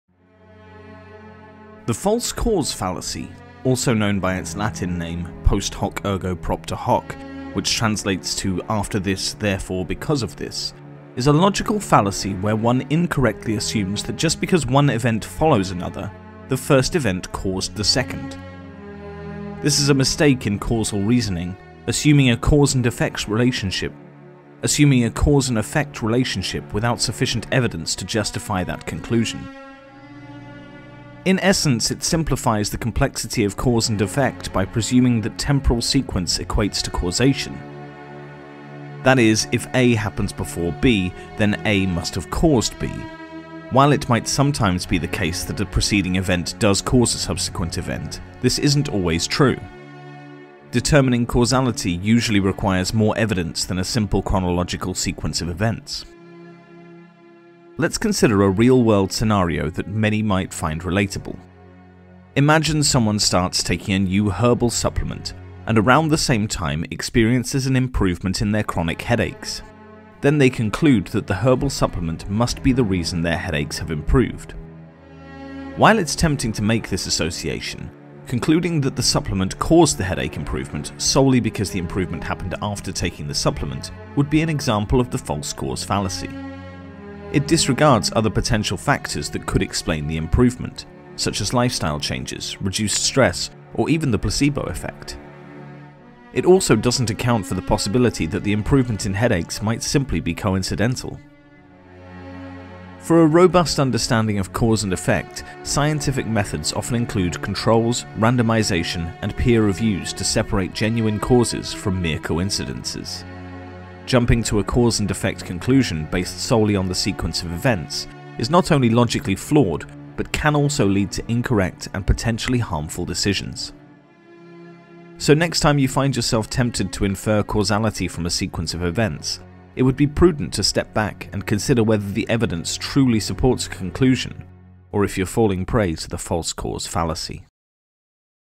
Logical Fallacies: The False Cause
The False Cause fallacy, also known by its Latin name "Post Hoc Ergo Propter Hoc" (which translates to "after this, therefore because of this"), is a logical fallacy where one incorrectly assumes that just because one event follows another, the first event caused the second. This is a mistake in causal reasoning, assuming a cause-and-effect relationship without sufficient evidence to justify that conclusion.

In essence, it simplifies the complexity of cause and effect by presuming that temporal sequence equates to causation. That is, if 'A' happens before 'B,' then 'A' must have caused 'B.' While it might sometimes be the case that a preceding event does cause a subsequent event, this isn't always true. Determining causality usually requires more evidence than a simple chronological sequence of events.

Example:
Let's consider a real-world scenario that many might find relatable. Imagine someone starts taking a new herbal supplement and around the same time experiences an improvement in their chronic headaches. They then conclude that the herbal supplement must be the reason their headaches have improved.

While it's tempting to make this association, concluding that the supplement caused the headache improvement solely because the improvement happened after taking the supplement would be an example of the False Cause fallacy. It disregards other potential factors that could explain the improvement, such as lifestyle changes, reduced stress, or even the placebo effect. It also doesn't account for the possibility that the improvement in headaches might simply be coincidental.

For a robust understanding of cause and effect, scientific methods often include controls, randomization, and peer review to separate genuine causes from mere coincidences. Jumping to a cause-and-effect conclusion based solely on the sequence of events is not only logically flawed but can also lead to incorrect and potentially harmful decisions.

So, the next time you find yourself tempted to infer causality from a sequence of events, it would be prudent to step back and consider whether the evidence truly supports such a conclusion or if you're falling prey to the False Cause fallacy.